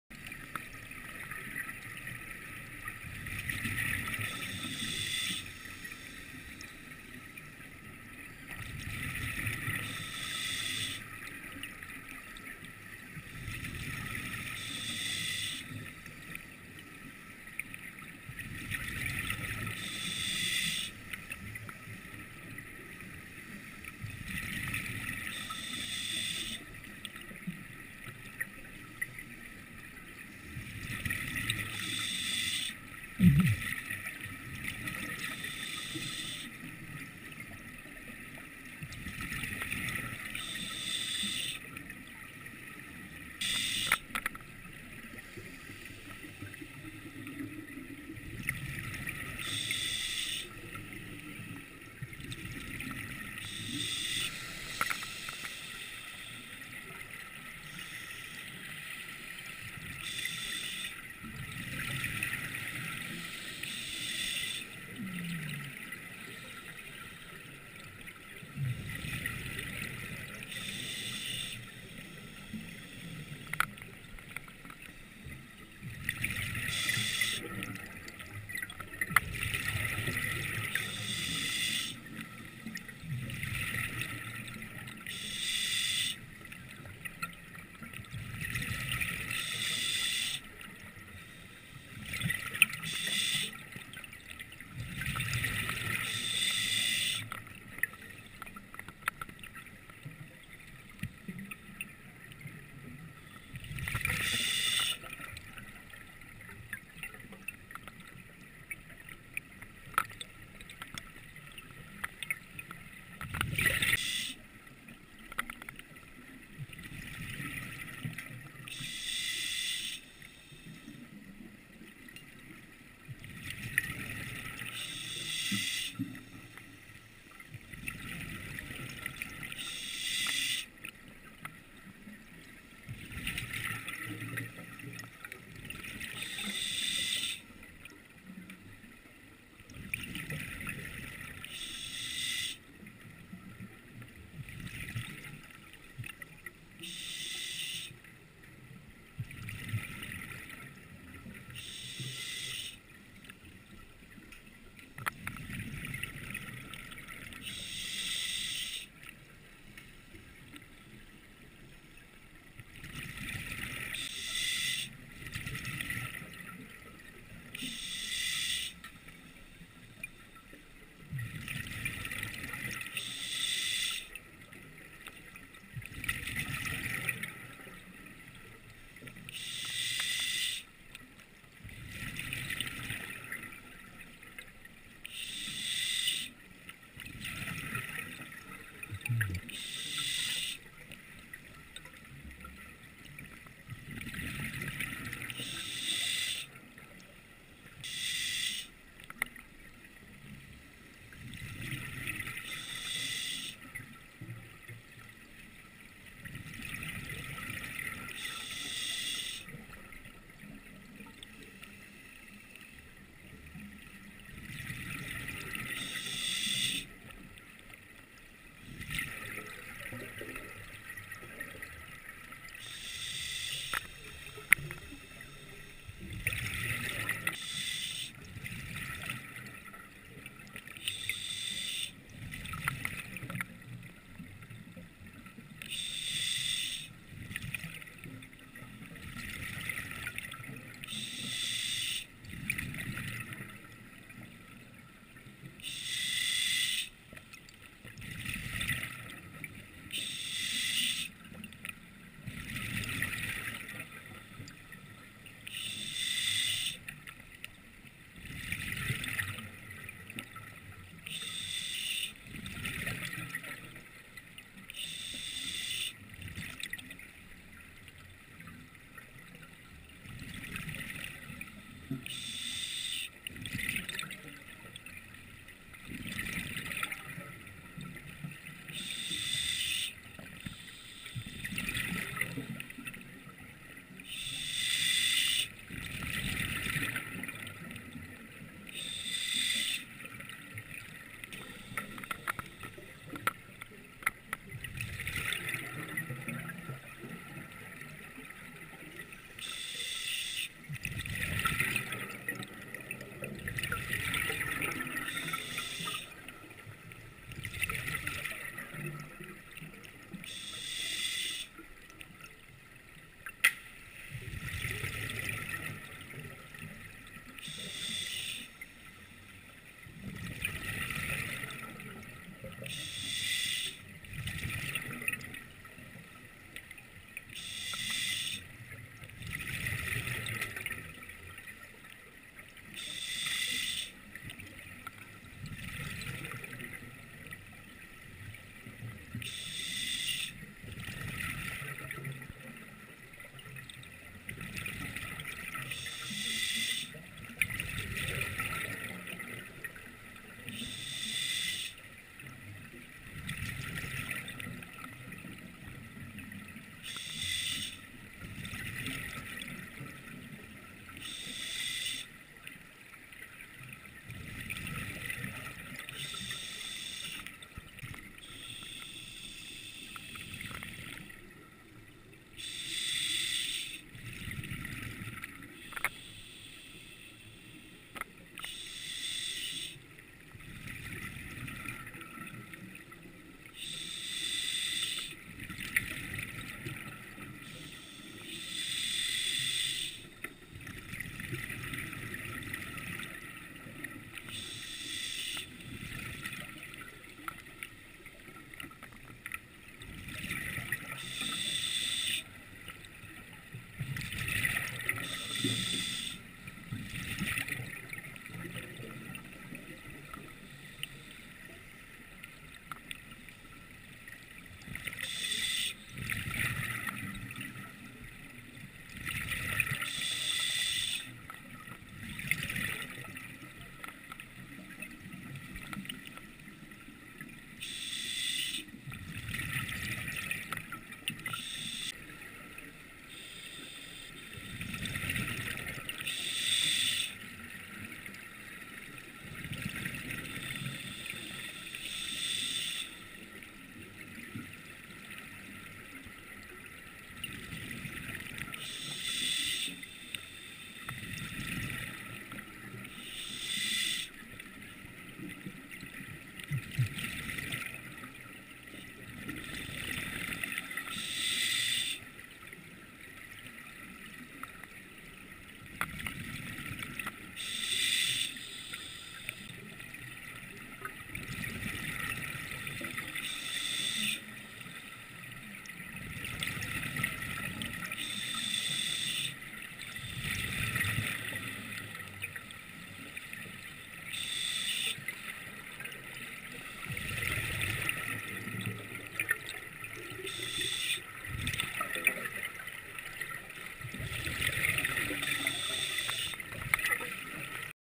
Nordstjerna 1.7.2013 (8:17 min)
Park Nordstjerna sunk in 1809 to 18-23m depth south of  Borstö island which is south of Turku. Most visible parts are the rudder and the huge anchor. The other anchor was raised and is still in Borstö. (NBA 1650)